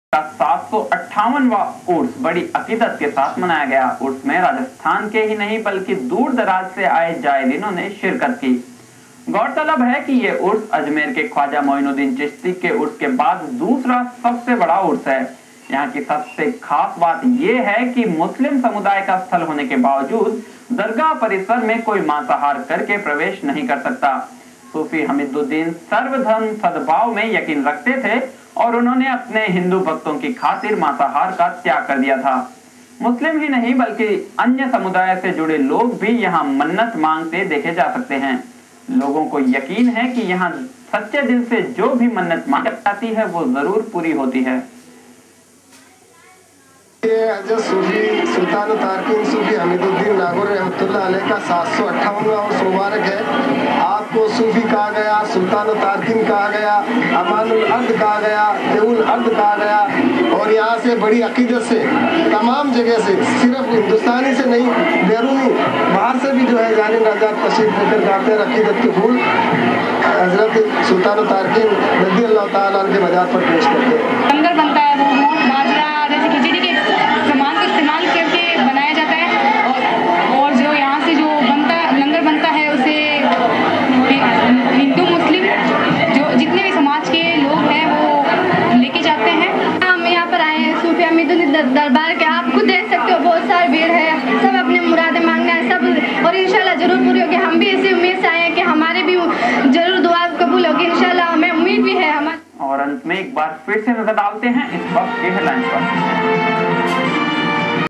01 news urs tv99.wmv
Nagaur (Rajasthan) Dargah Hamidudin Nagouri ka Salana Urs.